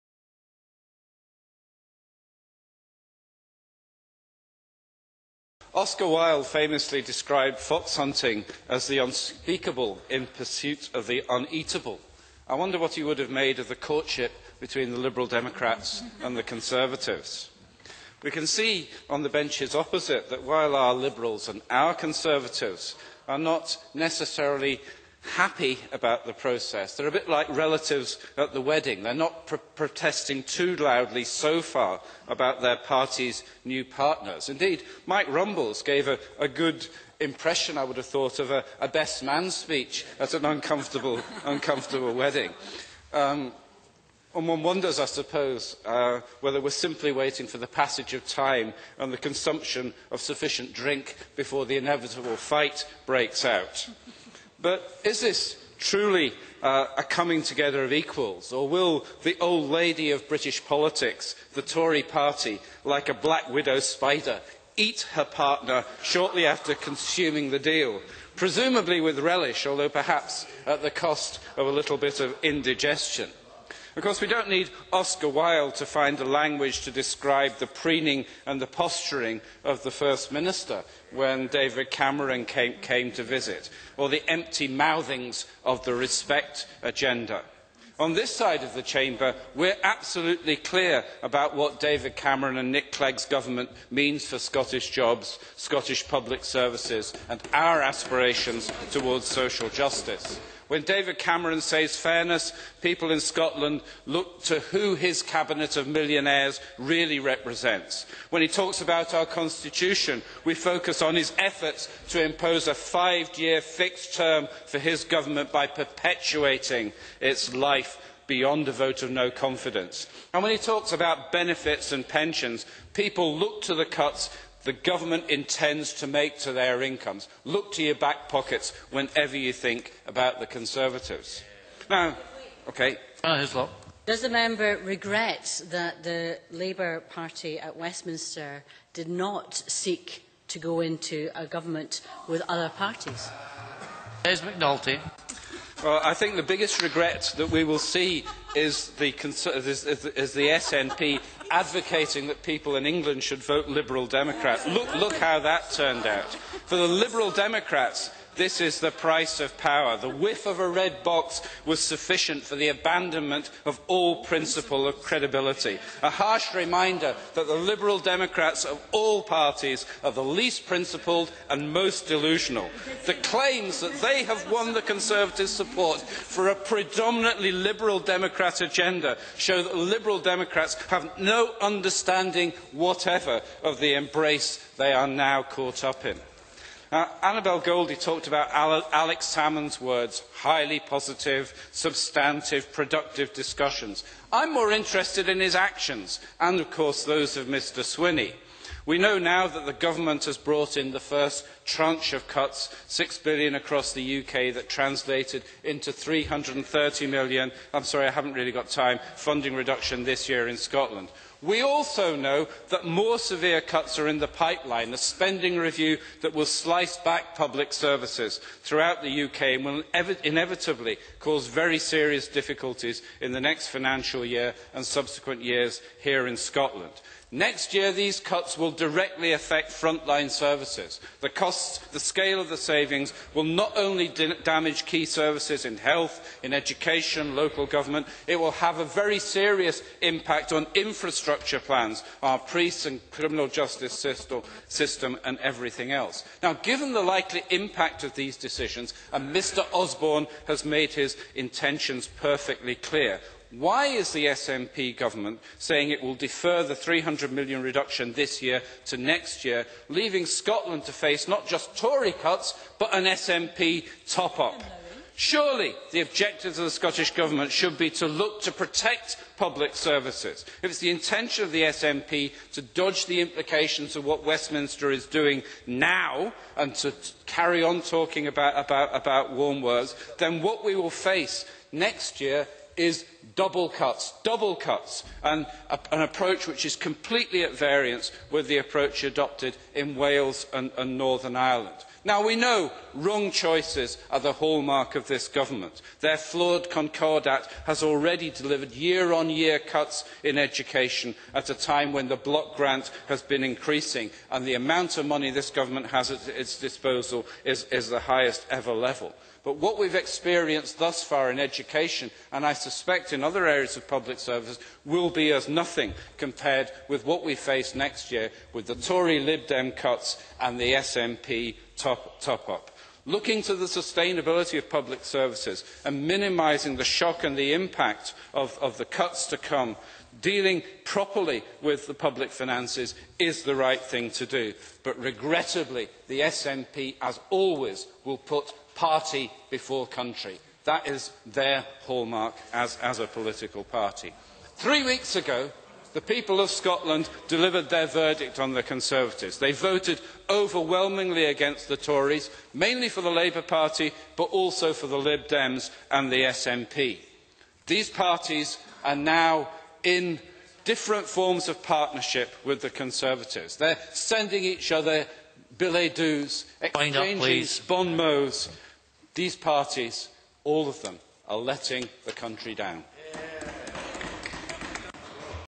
Her Majesty's Government (Relations)
Des debates relations between the UK Government and the Scottish Government. Looks particularly at the Westminster Coalition between the Conservatives and Liberal Demoncrats and the decision of the Scottish Government to defer its £330million cuts to 2011.

Courtesy of Holyrood TV